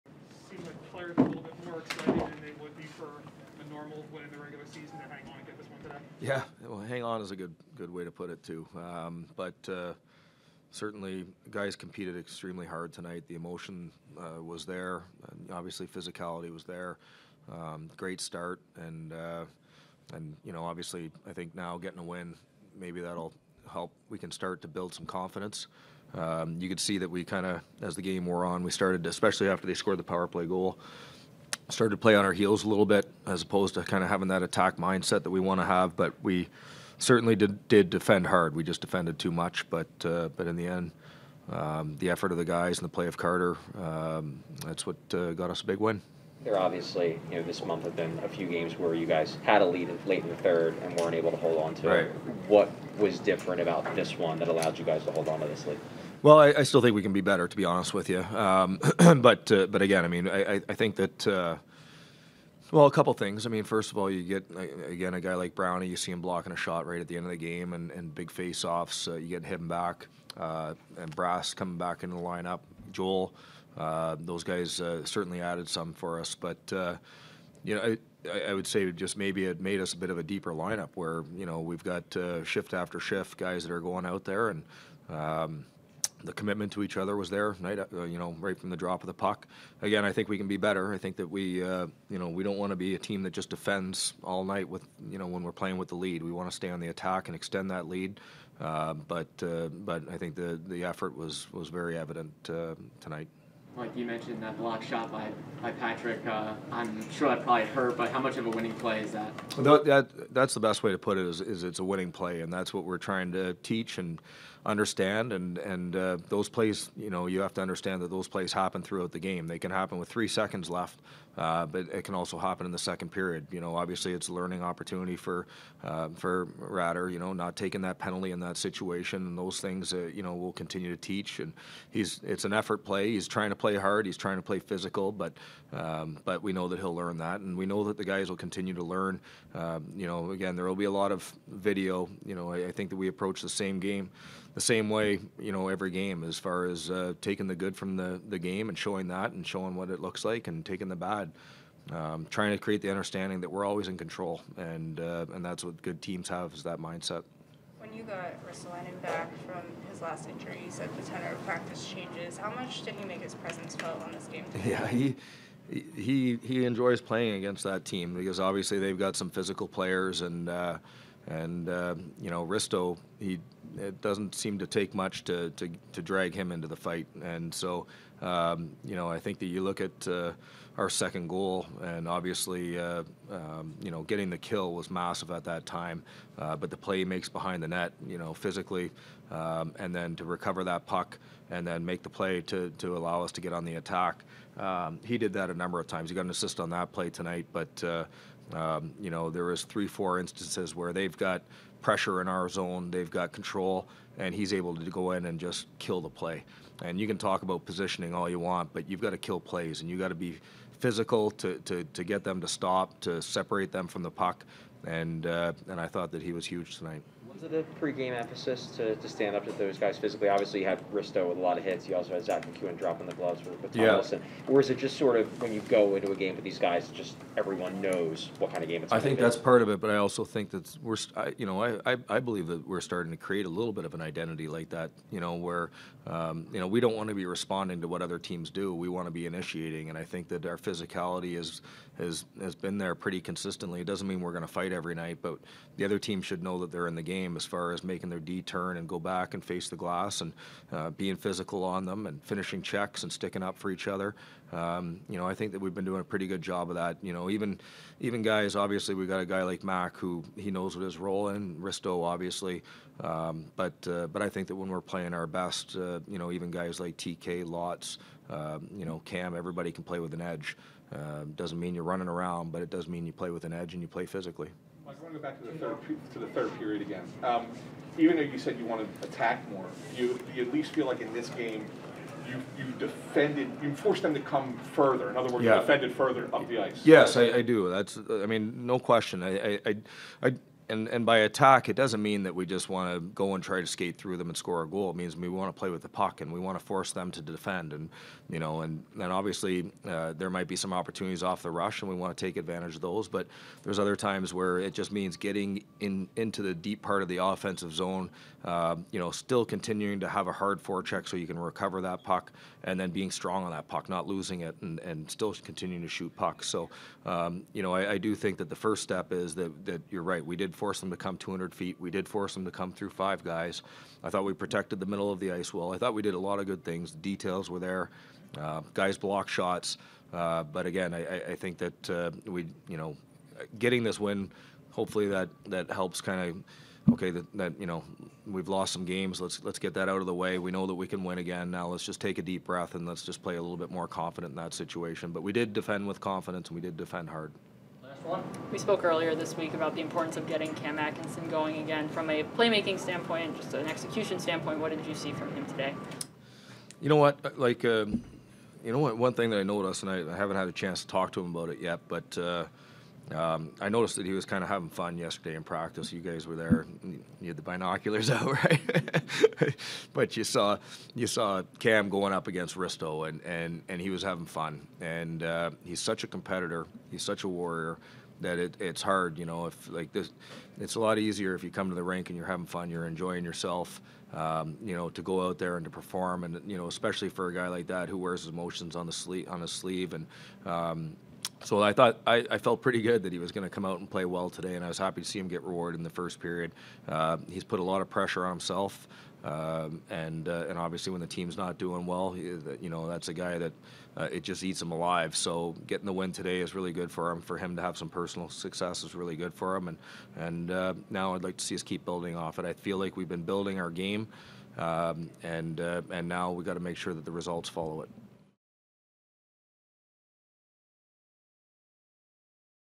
2/26 Flyers Postgame: Yeo
Hear from Mike Yeo following win over WSH.